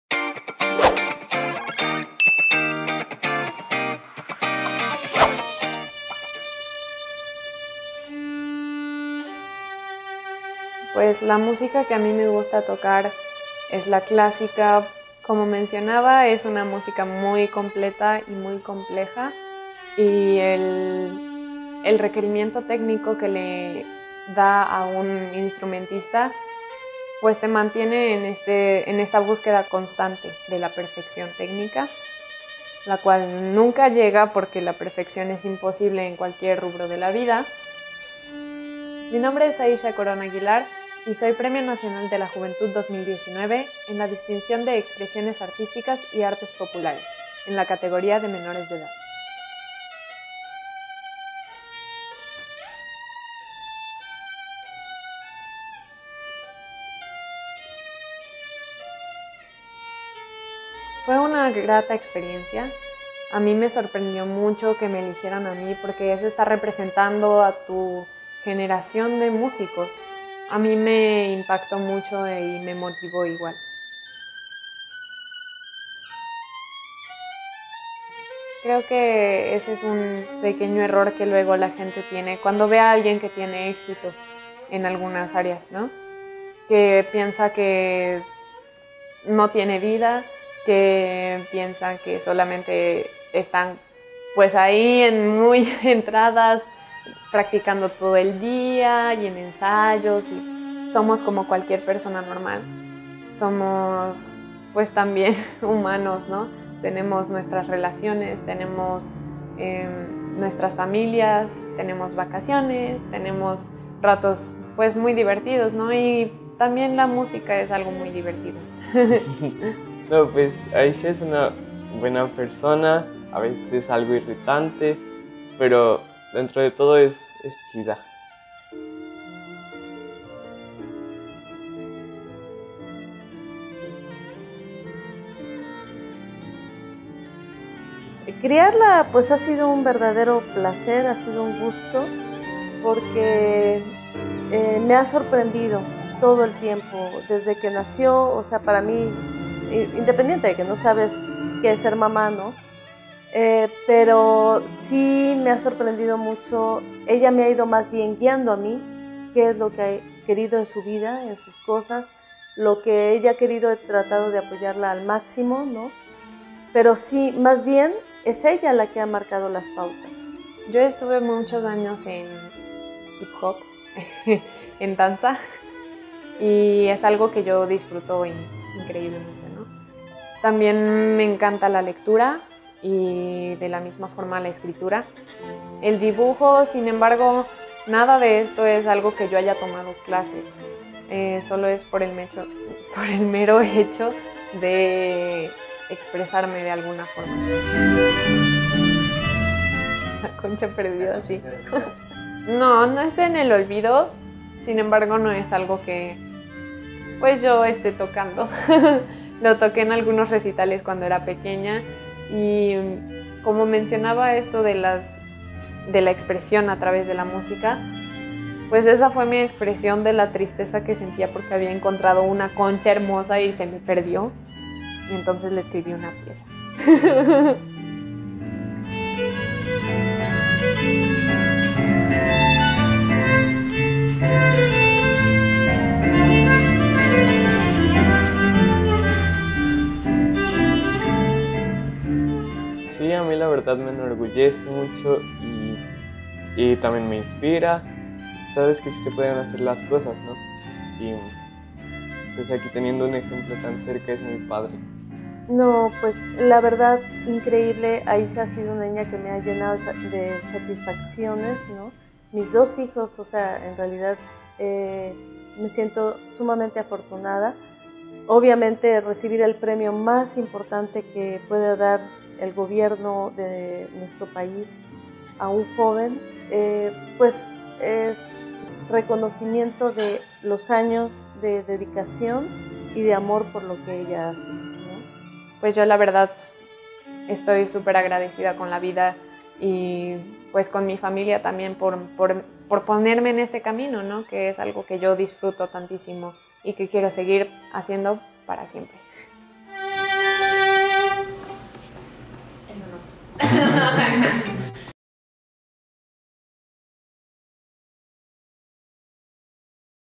Conociendo más sobre Aisha Corona, premio Nacional de la Juventud 2019
Desde muy joven, Aisha Corona, demostró su gusto por el violín, instrumento que le llevaría debutar como solista a los seis años de edad. Muchos han sido los éxitos y logros de Aisha, pero sin duda uno de los más grandes fue haber recibido el premio Nacional de la juventud 2019. Xalapeña, veracruzana y una joven destacada